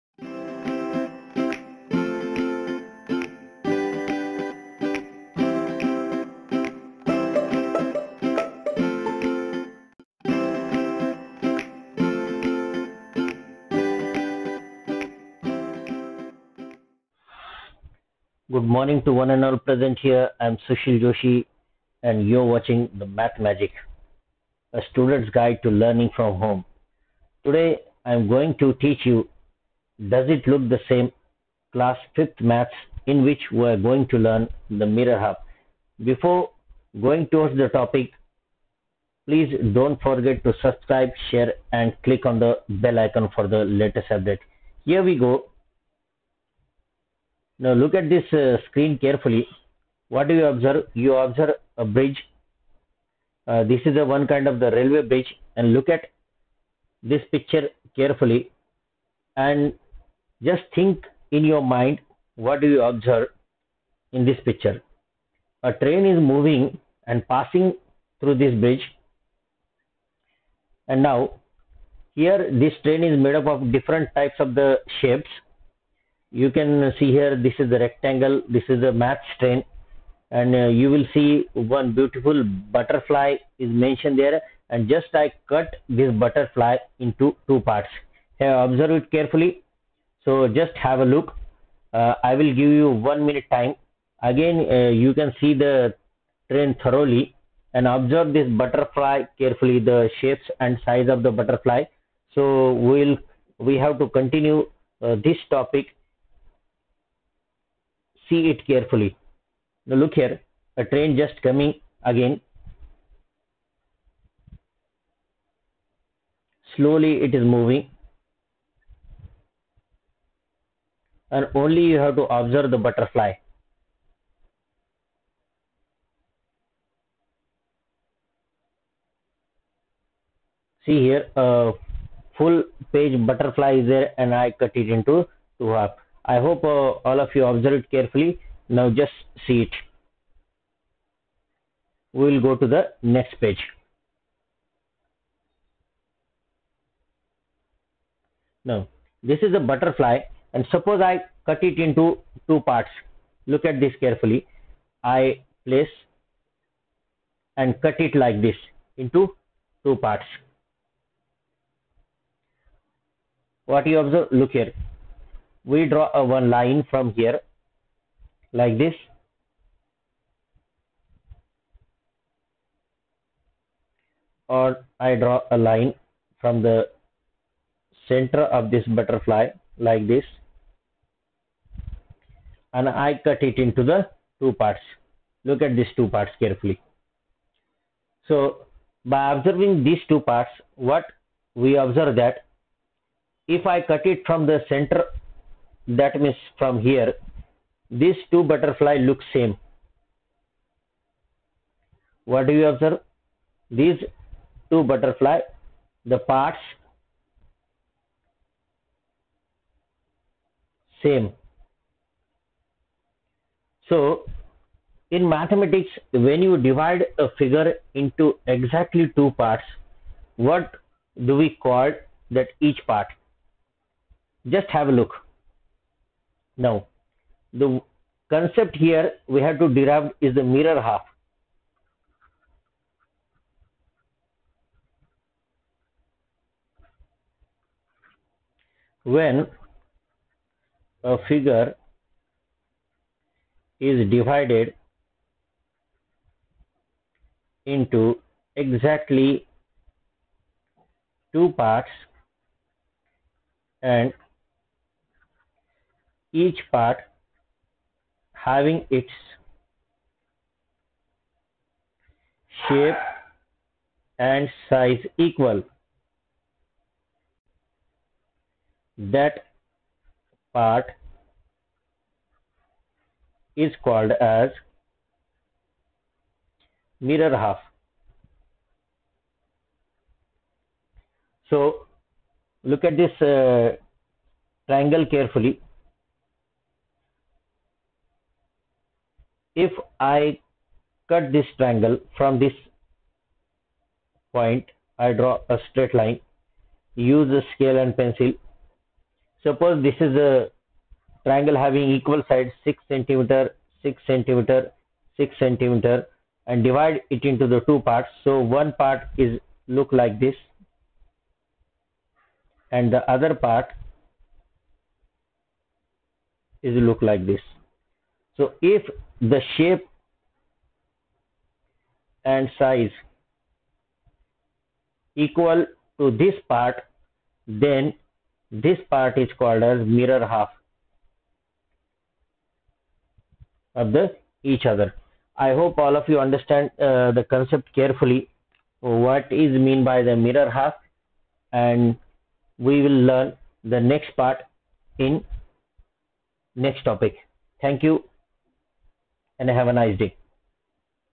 MIRROR HALF (DOES IT LOOK THE SAME PART 1)
MIRROR HALF (DOES IT LOOK THE SAME PART 1). In this topic we learn how to find the mirror half of any object.